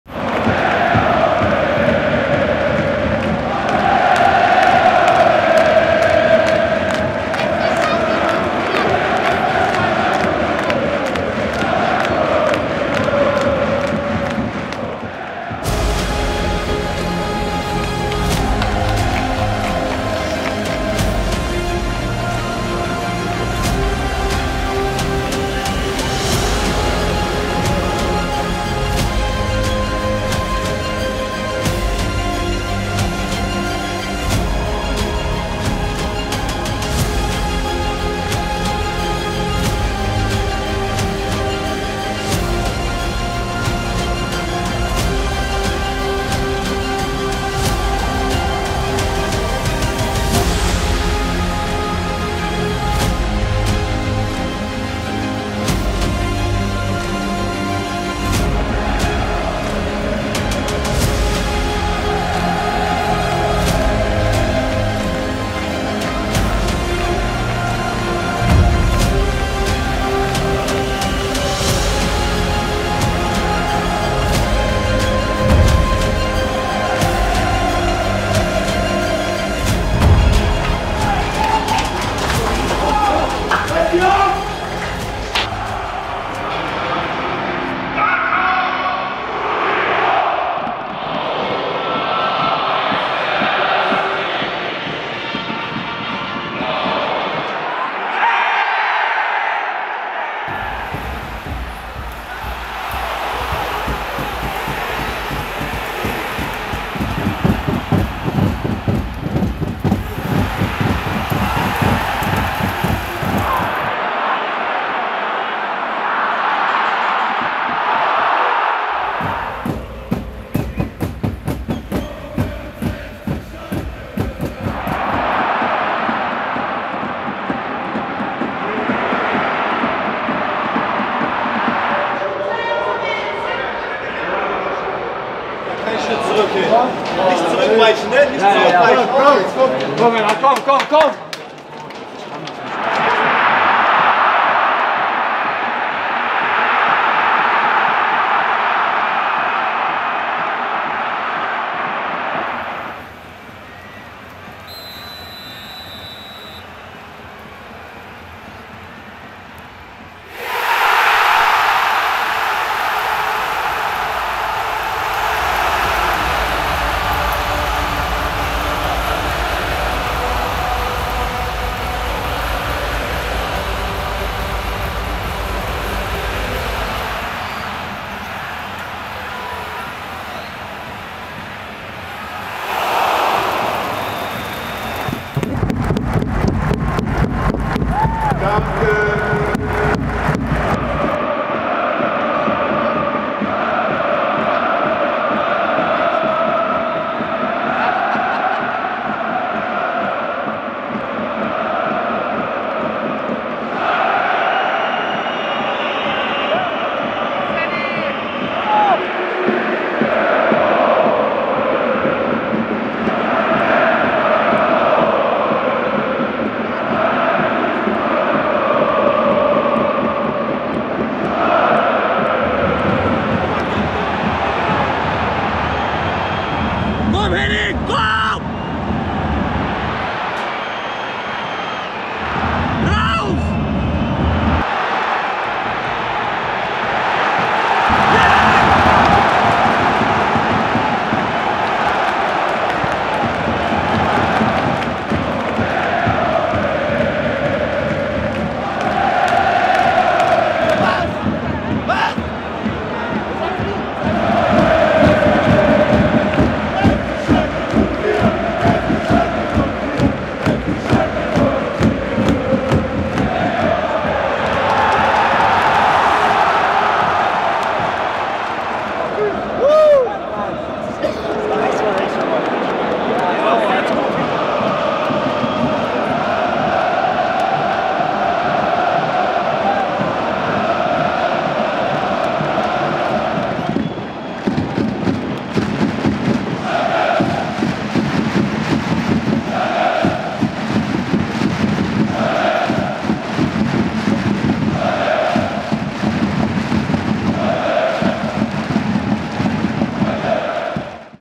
Access ALL AREAS | Churlinov singt Schalke-Lieder | FC Schalke 04 - SV Wehen Wiesbaden 1:0
Heimsieg für die Knappen! Kenan Karaman behält vom Elfmeterpunkt die Nerven und sorgt für die nächsten wichtigen drei Punkte für Königsblau. Welche Emotionen der Sieg bei den Protagonisten auf und neben dem Platz ausgelöst hat, seht ihr in der neuen Folge von "Access All Areas". Viel Spaß mit dem Blick hinter die Kulissen!